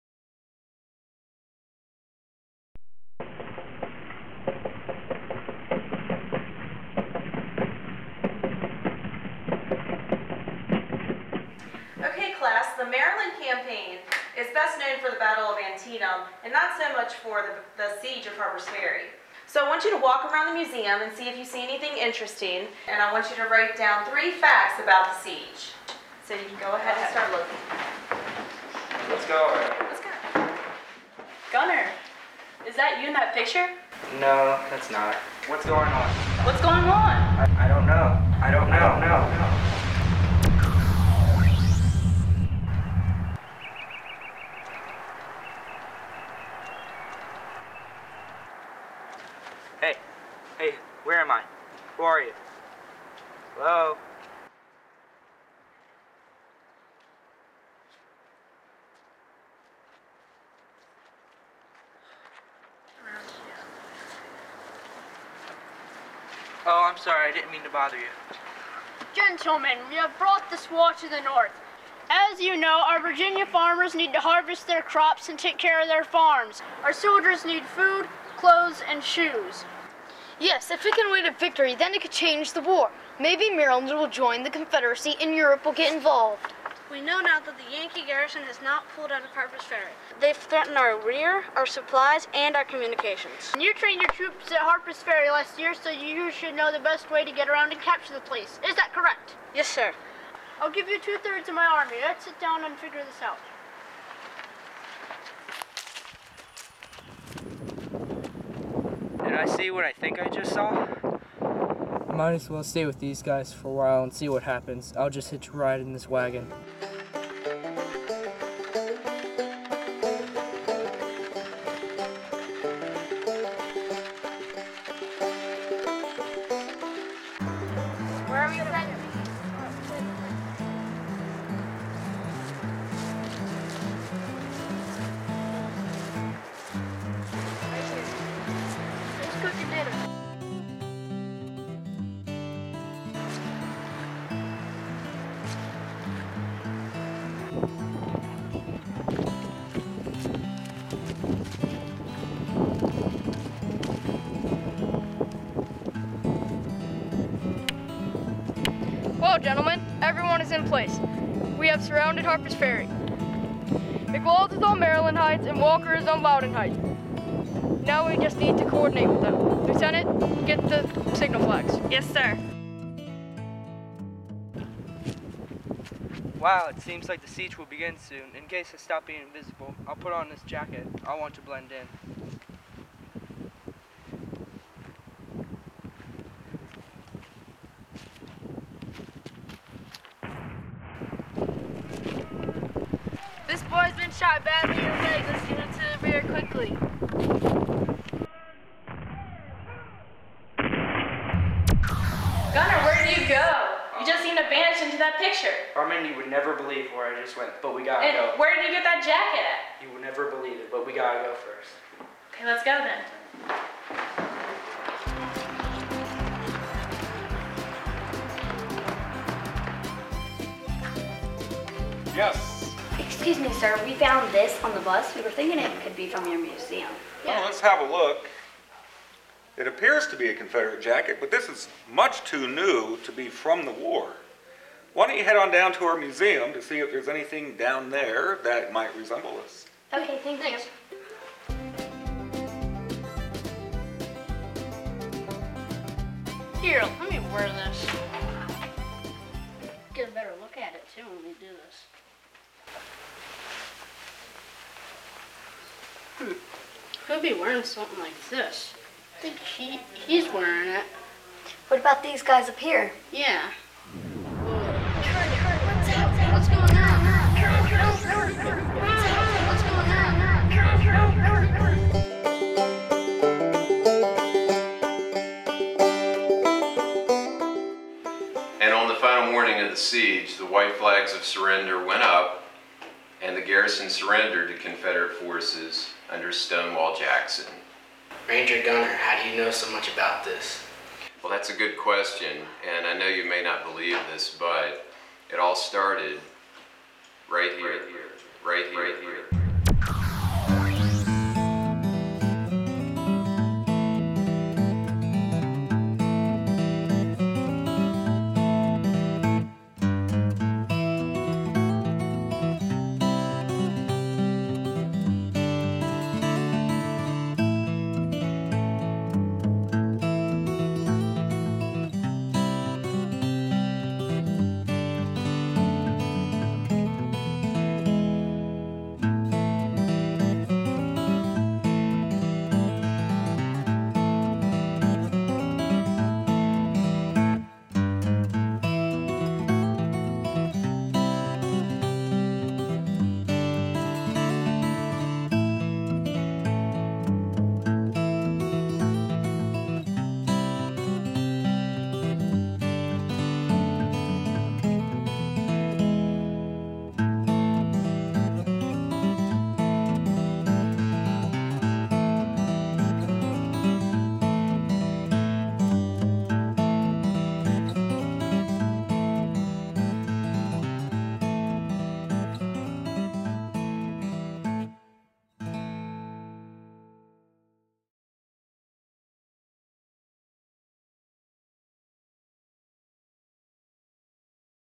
Maryland Campaign
In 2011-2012, Harpers Ferry National Historical Park continued its partnership with Harpers Ferry Middle School to produce student-created videos about Harpers Ferry's history. This is the fourth year of the project, and this year the focus is on the year 1862.